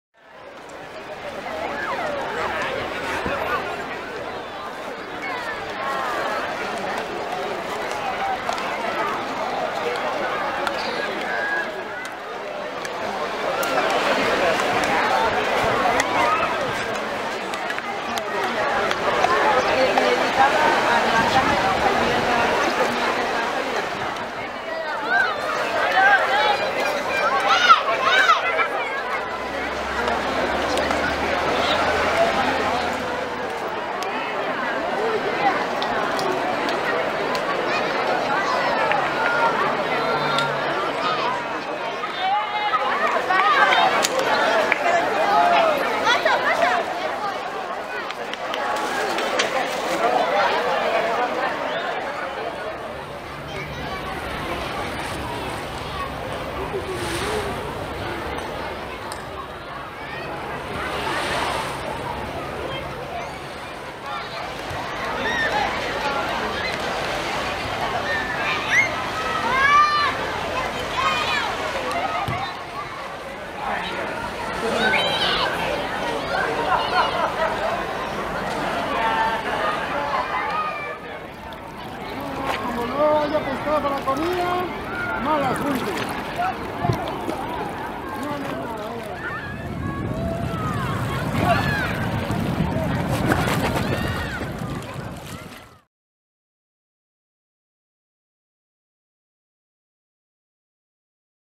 Disfrutando de la Semana Santa en la playa (13/04/2017)
Imágenes de este jueves santo en las playas de Torrevieja (Alicante) captadas por Joaquín Carrión y Rubén Torregrosa.

Las playas hoy están abarrotadas gracias al cielo despejado y a una temperatura máxima provisional de 23ºC.